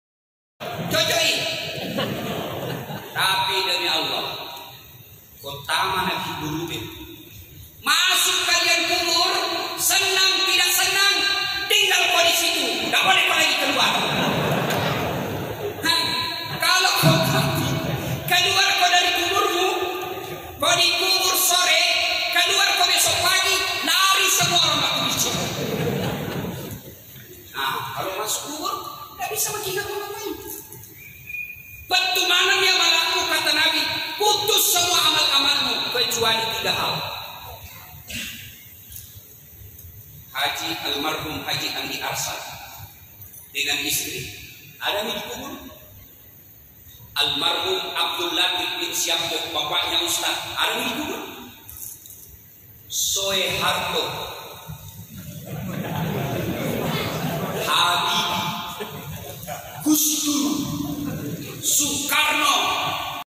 ustadz das'ad latief hadir di mesjid tanah bumbu
Lagi Mengawal Ustadz
Dr.H.Das'ad Latief.S.Sos,S.Ag,M.Si,Ph.D

Di Mesjid Ar-Raudhah Wardatul Arsyad
Jl.Lapangan 5 Oktober Kec.Simpang Empat Kab.Tanah Bumbu Kalimantan Selatan.